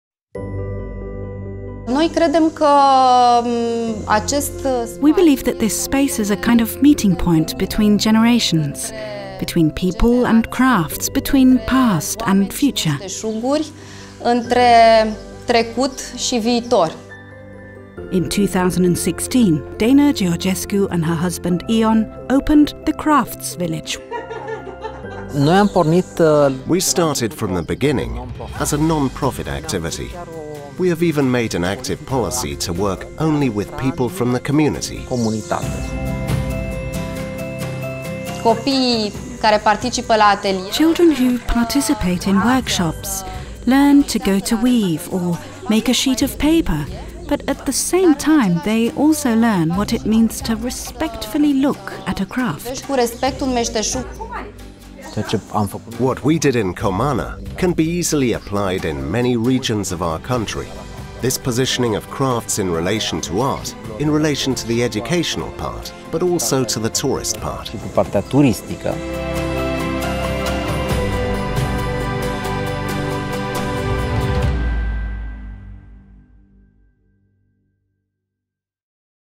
Satul Meşteşugurilor (versiune scurta)
„Credem că acest spațiu este un fel de punct de întâlnire între generații, între oameni și meșteșuguri, între trecut și viitor” spune Dana Georgescu, Comana Crafts Village

Descoperă secretele succesului în bioeconomie cu acest caz de afaceri rural inovator premiat!

Situat la poalele parcului natural Comana din România, satul meșteșugăresc combină grija pentru mediu, arte, educație și antreprenoriat - jucând un rol important în protejarea meșteșugurilor tradiționale rurale care altfel ar dispărea.

Ambițioasa echipă fondatoare a lui Dana și Ion Georgescu încurajează utilizarea materialelor naturale, nepoluante și bio-bazate, generează locuri de muncă locale și atrage vizitatori în comunitatea rurală locală. Vedeți cum combină tradiția cu inovația pentru a educa generațiile viitoare!